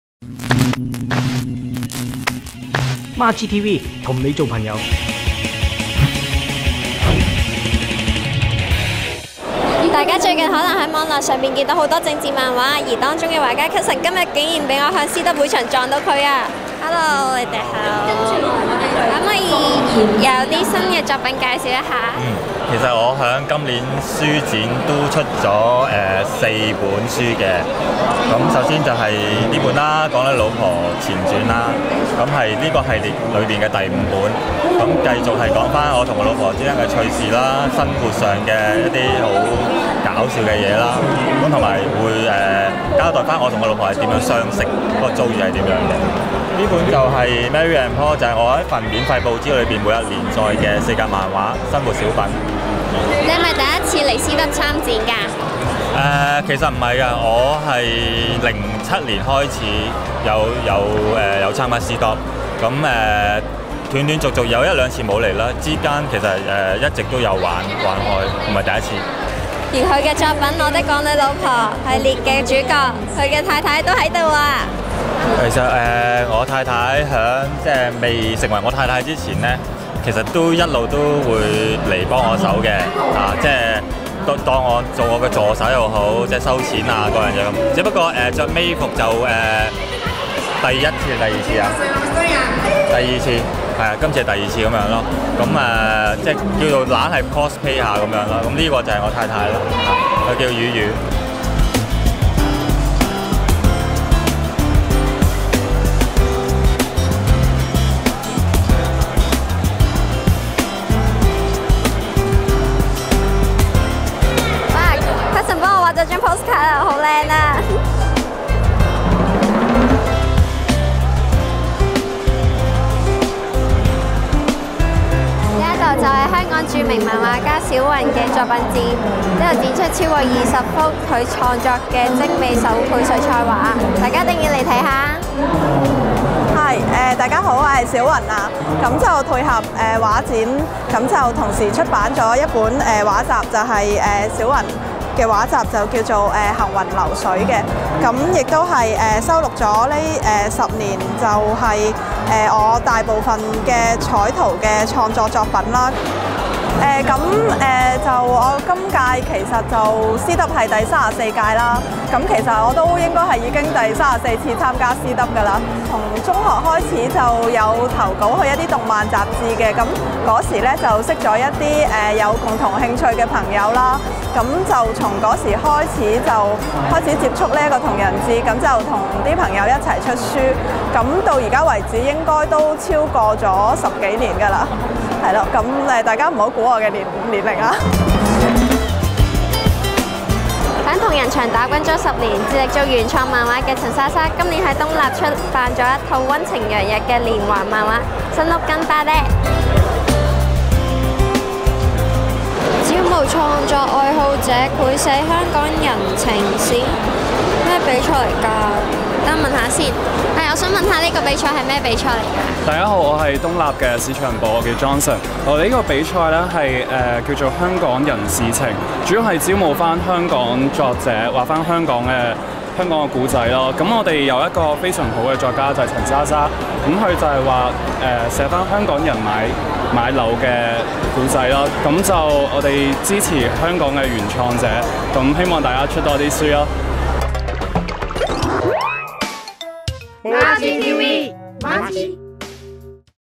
[Cosplay 同人區](5) - Comic World 番外篇 ( 08 OCT 2012 )
上一集就介紹左CW既動漫商品，今集就由小和親身專訪埸內著名漫畫家Cuson Lo、小雲和塵沙沙，佢地既作品各有特色，漫畫迷唔好錯過呀!!!~

LG & Samsung 智能電視用戶Smart TV App：Majitv